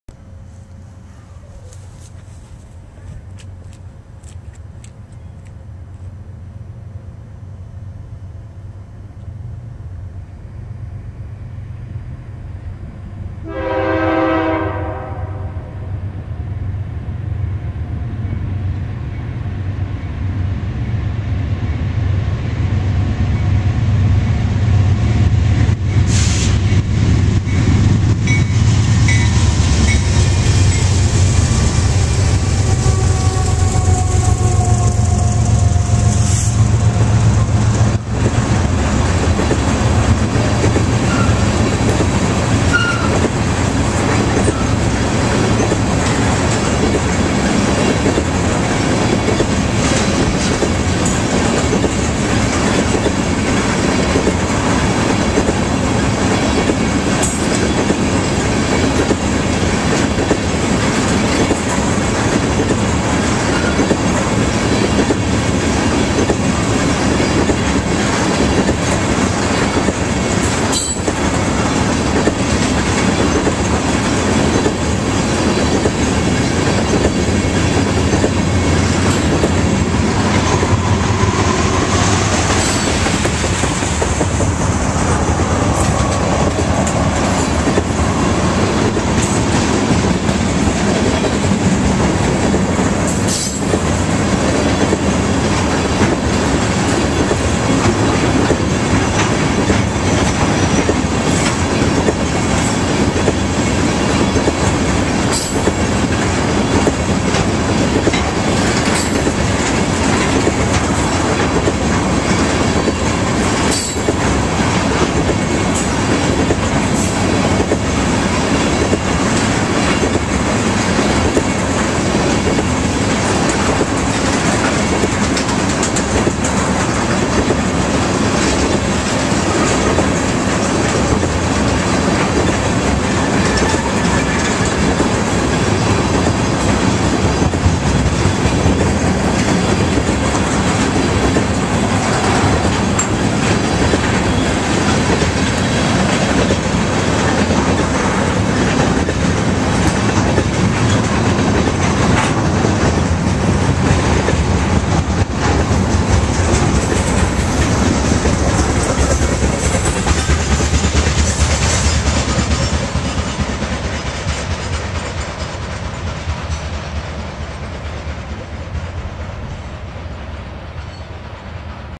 CSX K038 ROLLS WEST THRU YARDLEY PA BNSF TRIO
4/25/15 Here we have CSX K038, A loaded crude oil train as it passes west through Yardley PA with a BNSF trio for power.
Power: BNSF 5405 C44-9W, 5204 C44-9W, 5894 ES44AC